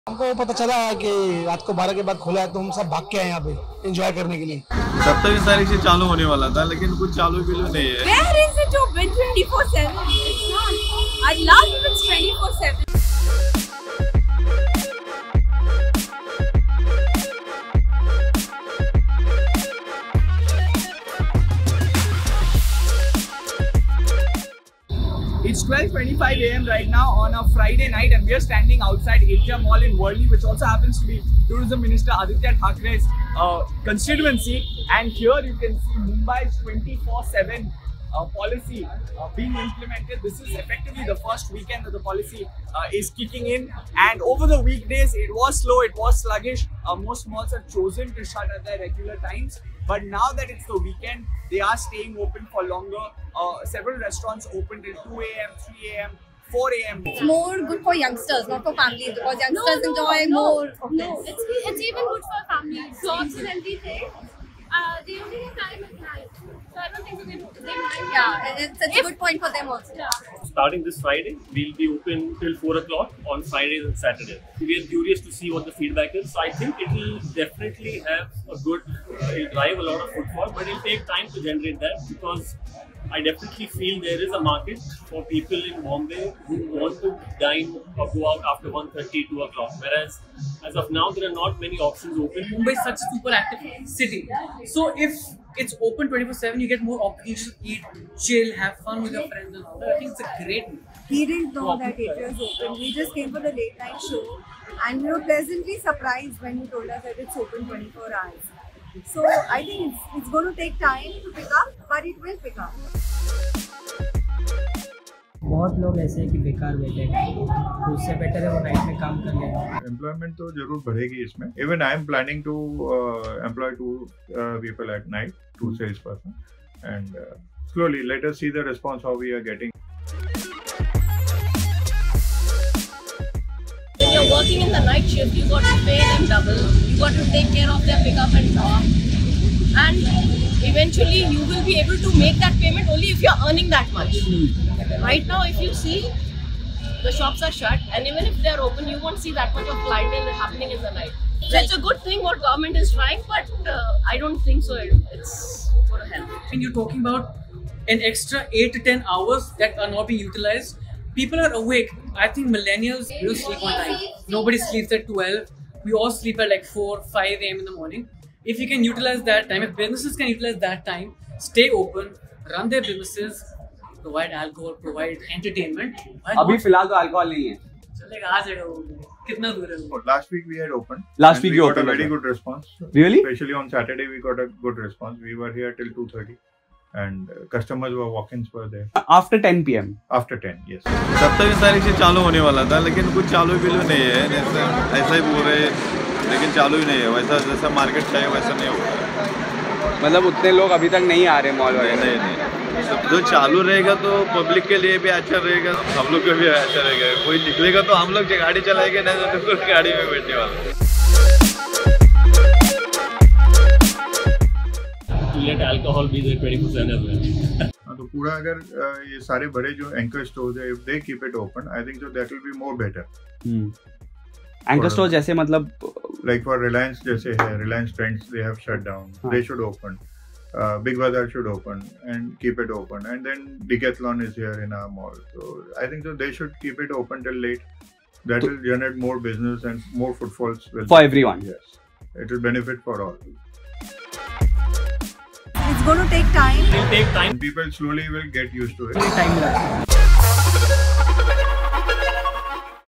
Mumbai After Midnight: Are People Giving a Thumbs up to New 24x7 Policy? | The Quint
Restaurants and malls in Mumbai are now allowed to stay open all night, but is the city giving a thumbs up or a thumbs down to its new 24x7 policy?